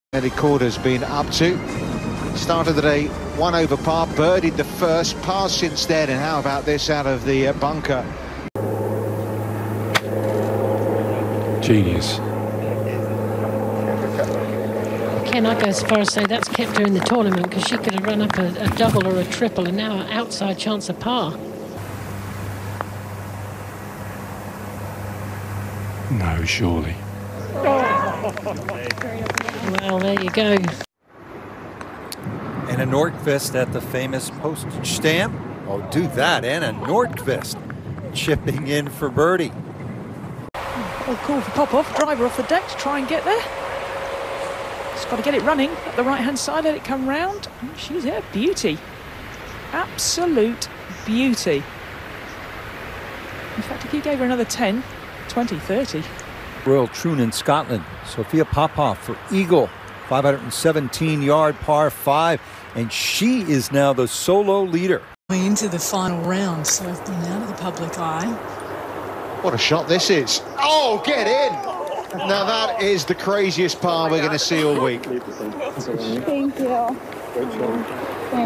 Top 5 Shots - 2020 AIG Women's Open
Check out the incredible top five shots from the 2020 AIG Women's Open.